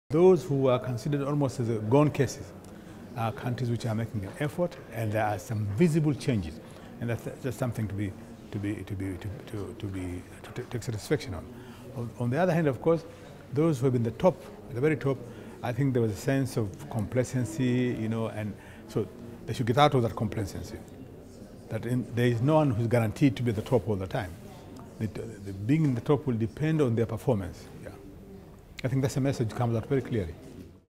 Salim Ahmed Salim: One of the key messages from the 2014 IIAG
Salim Ahmed Salim is one of Africa’s most senior diplomats and statesmen. He is also a member of the MIF board.